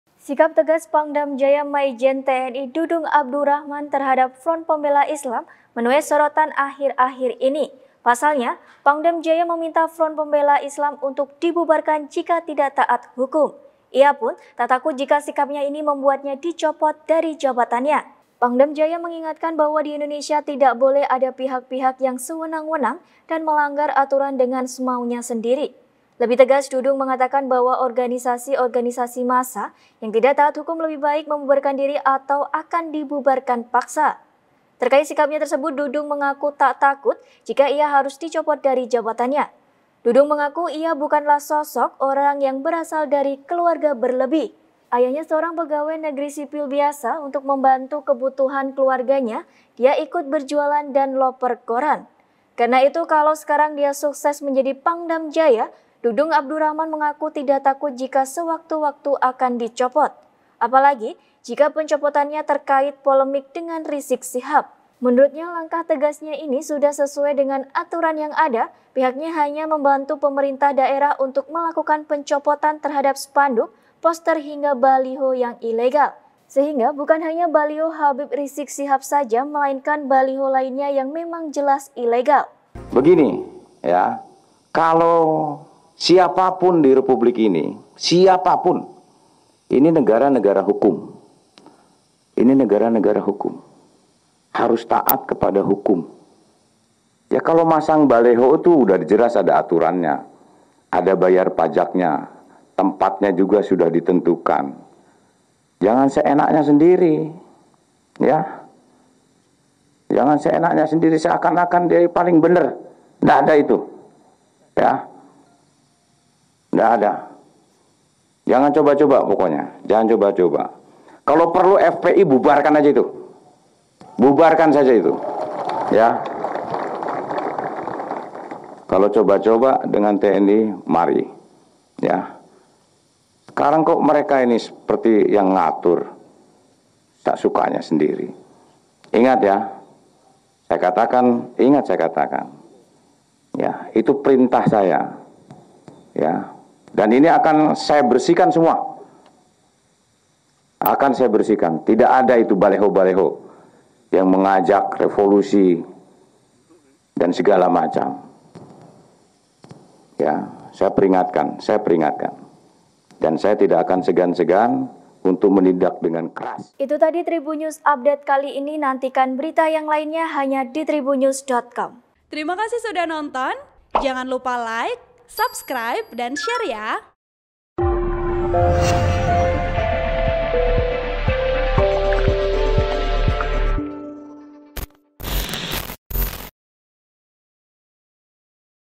Tegas Terhadap FPI hingga Ancam Bubarkan, Pangdam Jaya: Saya Tak Takut Dicopot
TRIBUN-VIDEO.COM - Sikap tegas Pangdam Jaya Mayjen TNI Dudung Abdurachman terhadap Front Pembela Islam menuai sorotan akhir-akhir ini.

Pangdam Jaya Mayjen TNI Dudung Abdurachman meminta Front Pembela Islam (FPI) untuk dibubarkan jika tidak taat hukum.
Ia pun tak takut jika sikapnya ini membuatnya dicopot dari jabatannya

Pangdam Jaya mengingatkan bahwa di Indonesia tidak boleh ada pihak-pihak yang sewenang-wenang dan melanggar aturan dengan semaunya sendiri.

Lebih tegas, Dudung mengatakan bahwa organisasi-organisasi massa yang tidak taat hukum lebih baik membubarkan diri, atau akan dibubarkan paksa.

Terkait sikapnya tersebut, Dudung mengaku tak takut jika ia harus dicopot dari jabatannya.

Dudung mengaku ia bukanlah sosok orang yang berasal dari keluarga berlebih.

Ayahnya seorang pegawai negeri sipil (PNS) biasa, untuk membantu kebutuhan keluarganya dia ikut berjualan dan loper koran.

Karena itu, kalau sekarang dia sukses menjadi Pangdam Jaya, Dudung Abdurahman mengaku tidak takut jika sewaktu-waktu akan dicopot.

Apalagi jika pencopotannya terkait polemik dengan Rizieq Shihab.

"Dulunya (saya) tukang koran. Jadi kalau saya jadi Pangdam (sudah) bersyukur banget dan Bapak saya cuma PNS. Jadi misalnya dicopot gara-gara ini, copot lah, saya nggak pernah takut, benar saya nggak takut," jelasnya di Makodam Jaya, Senin (23/11/2020).

Menurutnya, langkah tegasnya ini sudah sesuai dengan aturan yang ada.

Pihaknya hanya membantu pemerintah daerah untuk melakukan pencopotan terhadap spanduk, poster hingga baliho yang ilegal.

Sehingga bukan hanya baliho HRS saja melainkan baliho lainnya yang memang jelas ilegal.(*)

Artikel ini telah tayang di Wartakotalive dengan judul Mayjen TNI Dudung Abdurachman Nggak Takut Dicopot, Dulu Tukang Koran, Jadi Pangdam Sudah Bersyukur,
Artikel ini telah tayang di surya.co.id dengan judul Masa Lalu Pangdam Jaya Terkuak saat Polemik Pencopotan Baliho Rizieq Shihab: Saya Gak Takut Dicopot,